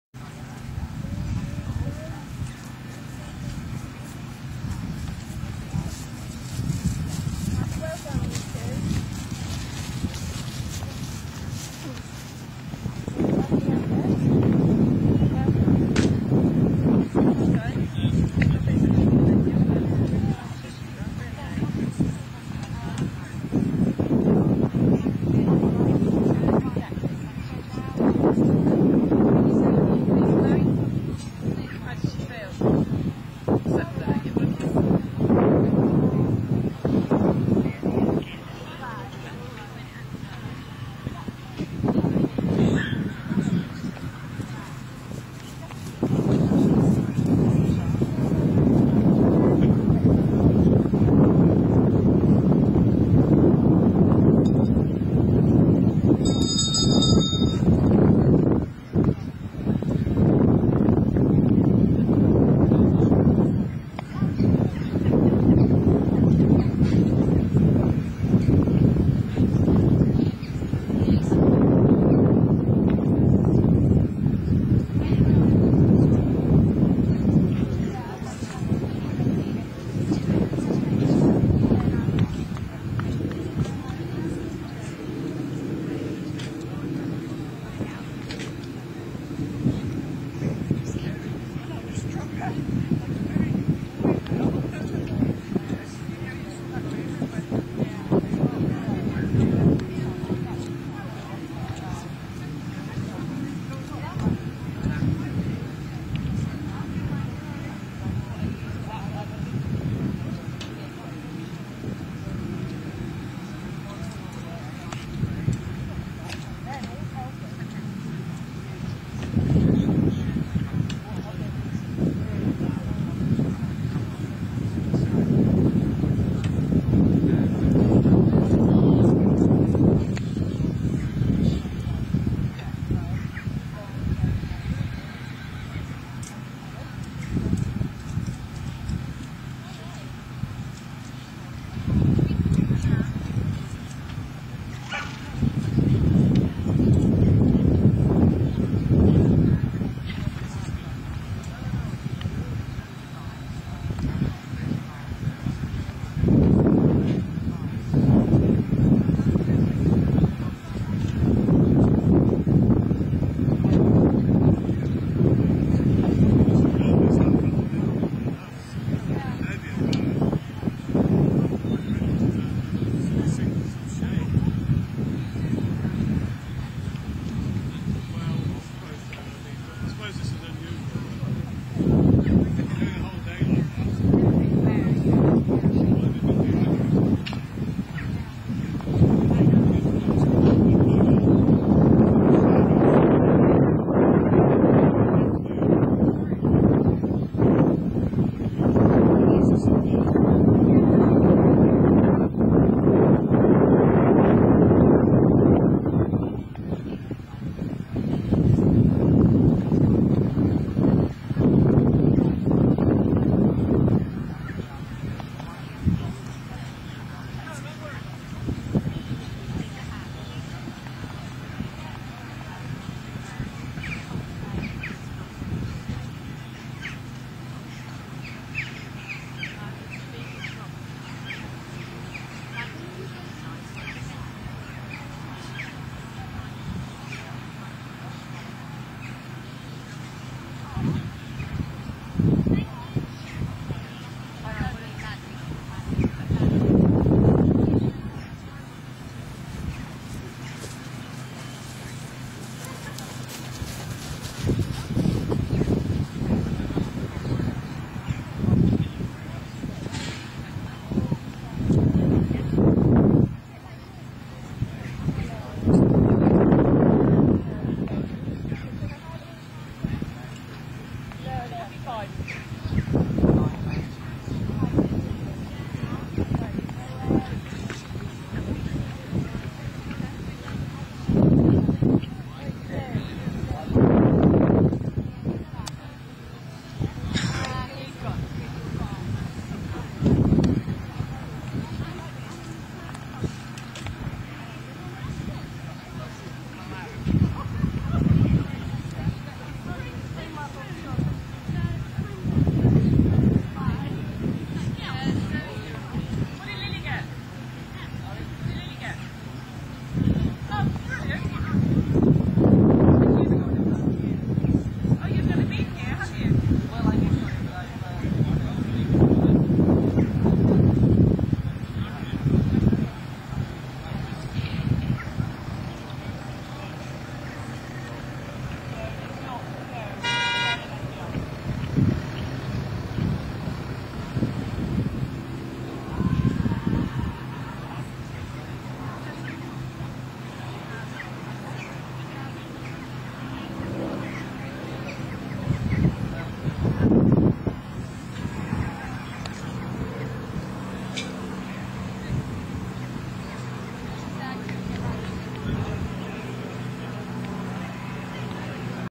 Brand Hall PT** Dressage
Monklands Boy - For Sale..       This is the dressage at Brand Hall Pony Championships 2011